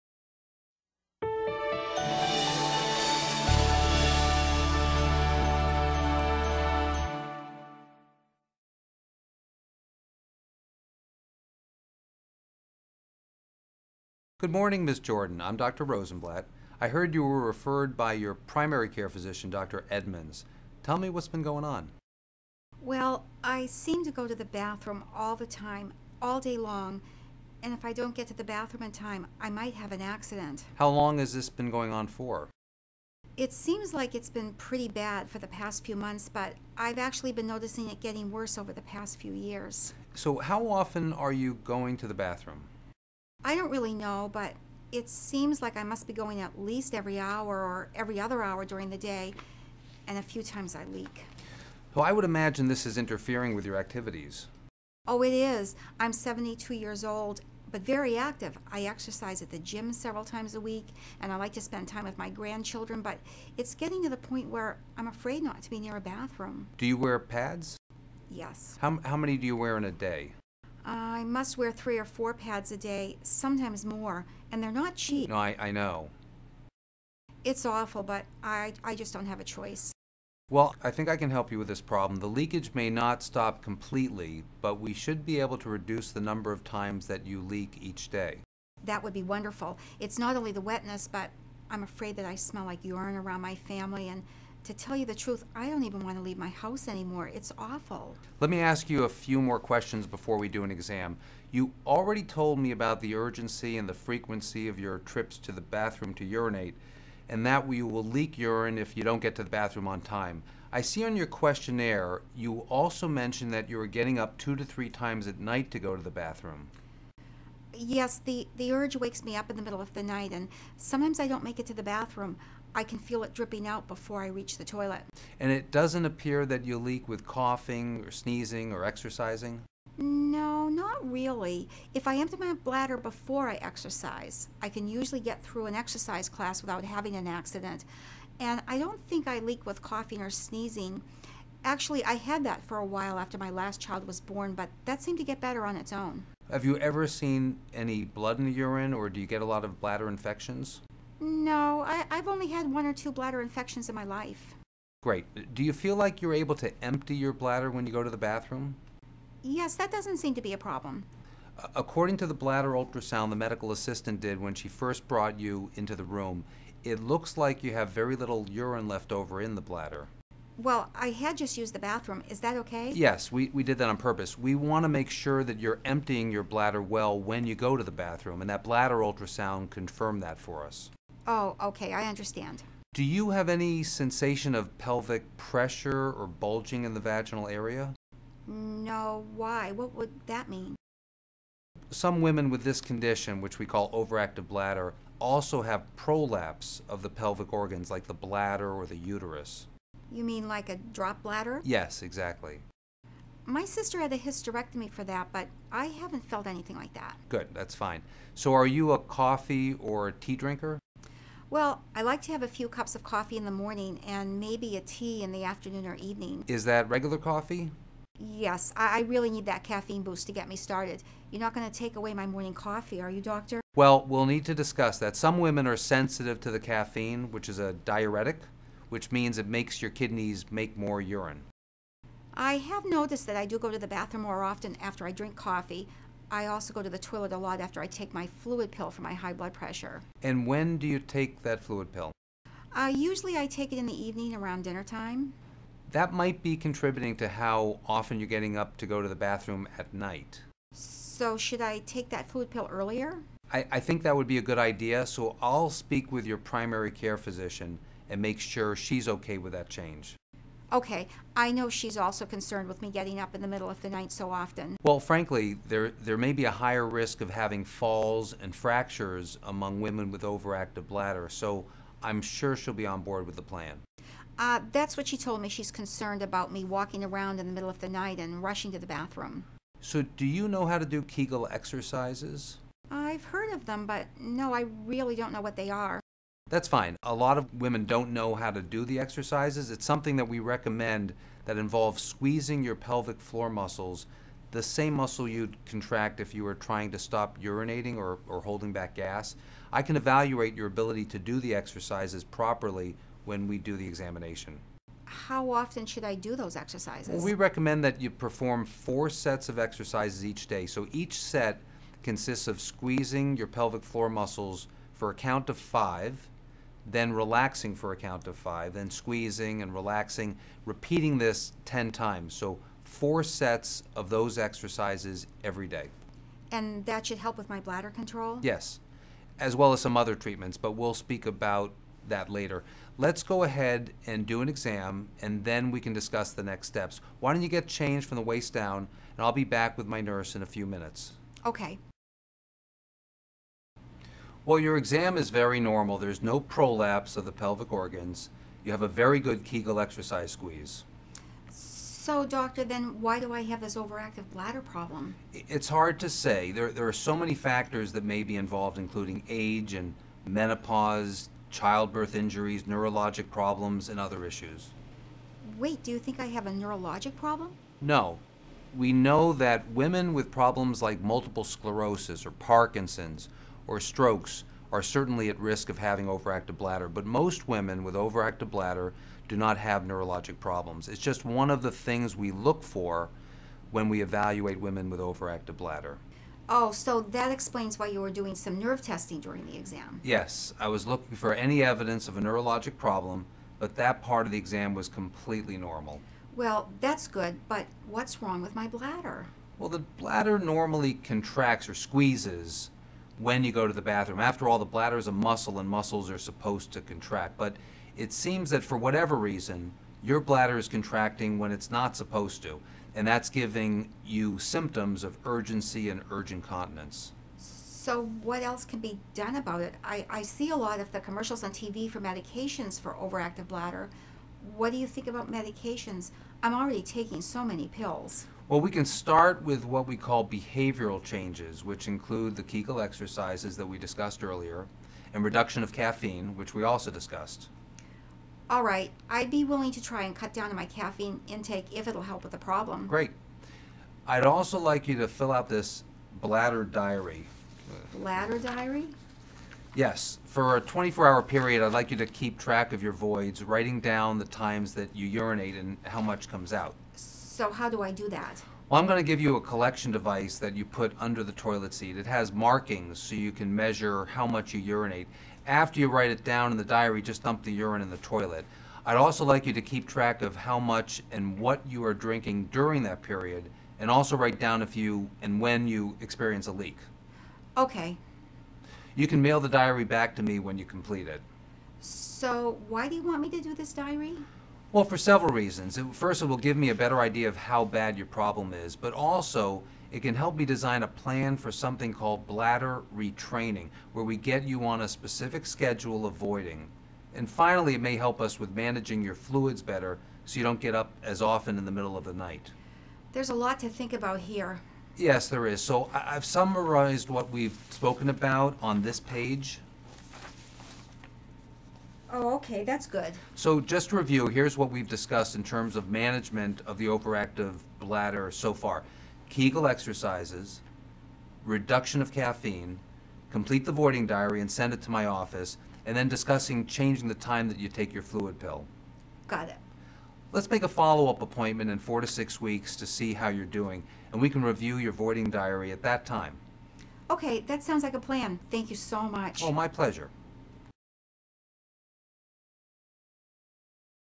Ms. Jordan’s First Visit with Dr. Rosenblatt
A patient (Ms. Jordan) with overactive bladder (OAB) has been referred by her primary care provider to an urogynecologist (Dr. Rosenblatt). This video documents the patient’s initial appointment with the specialist, including a discussion about her symptoms, starting Kegel exercises, and completing a voiding diary.